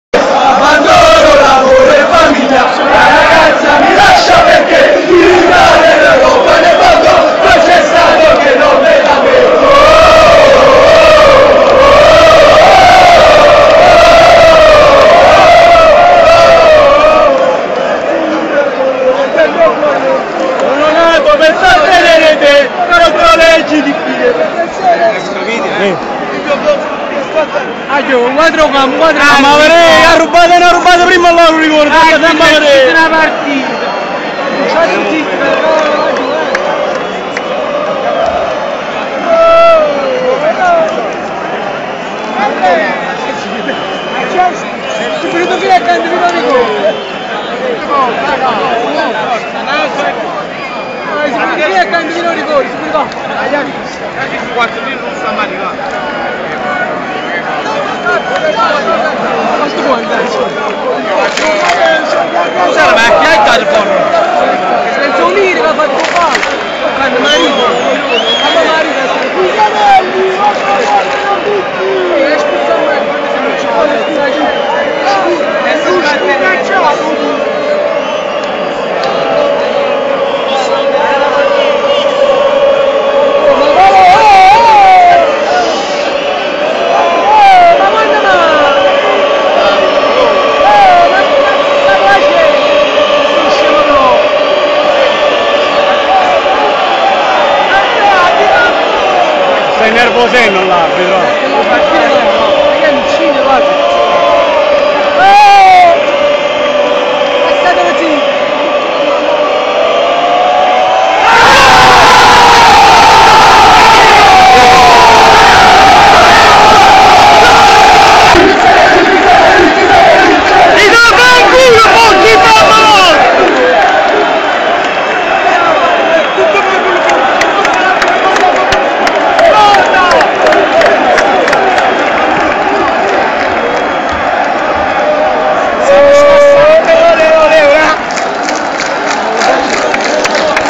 Parma Napoli 1 - 2 (20-04-2008) - Video #2
Prima trasferta vissuta col Napoli in serie A - Rigore di Domizzi live dal settore ospiti
A Parma affrontiamo una squadra che deve vincere per salvarsi, i partenopei non hanno nulla da perdere e dopo una gara combattuta con tante ammonizioni e diverse espulsioni, portano via dal Tardini 3 punti d'oro.
34^A giornata serie A 2007/08
PARMA-NAPOLI 1-2 (pt 1-1)
MARCATORI: 23' pt rig. Budan (P), 49' pt rig. Domizzi (N), 27' st Bogliacino (N)